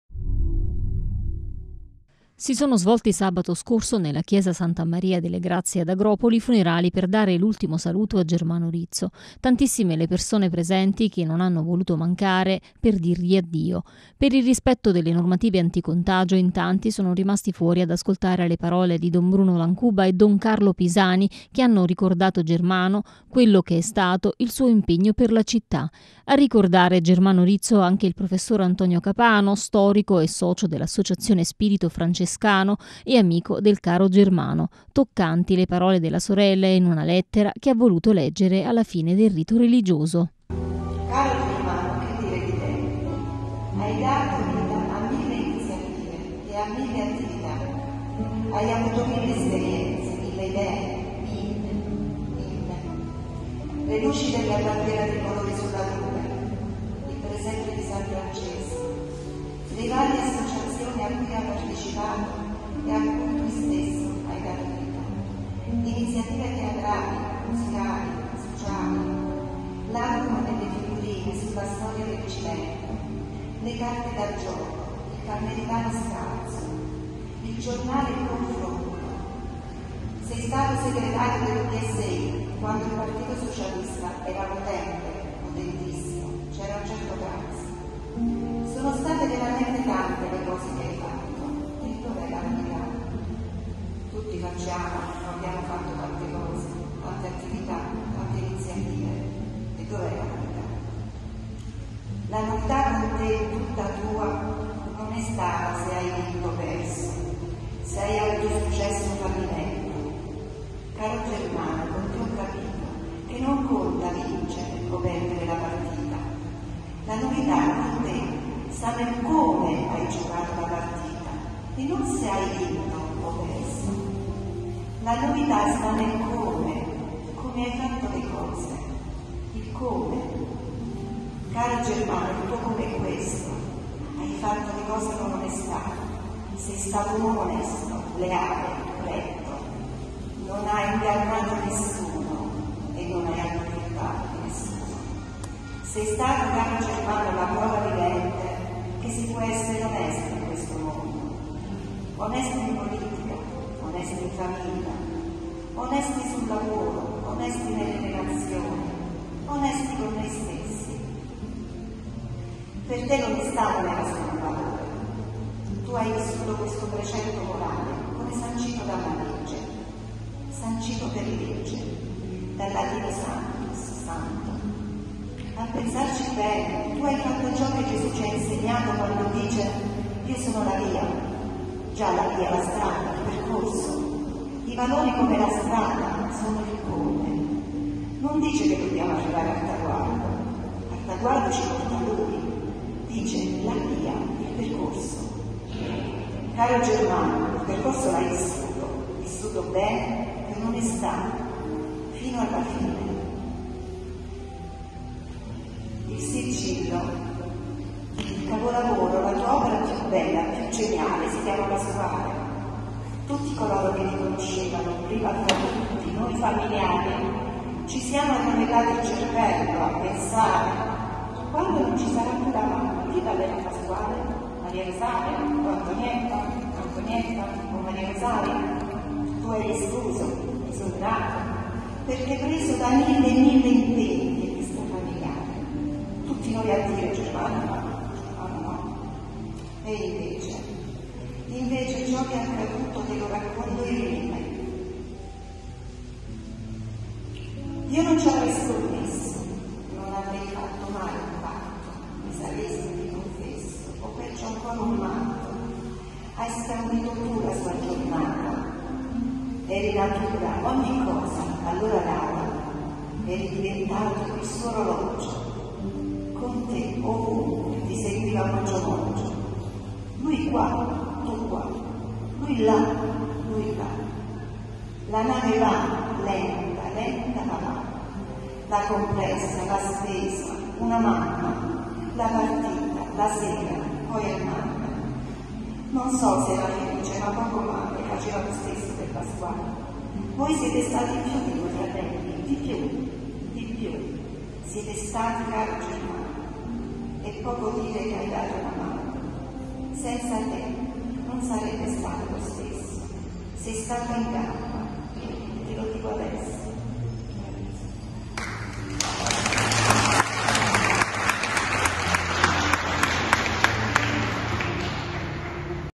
Agropoli, in tanti per l’ultimo saluto a Germano Rizzo
Si sono svolti sabato scorso nella chiesa S. Maria delle Grazie ad Agropoli i funerali per dare l’ultimo saluto a Germano Rizzo. Tantissime le persone presenti che non hanno voluto mancare per dirgli addio.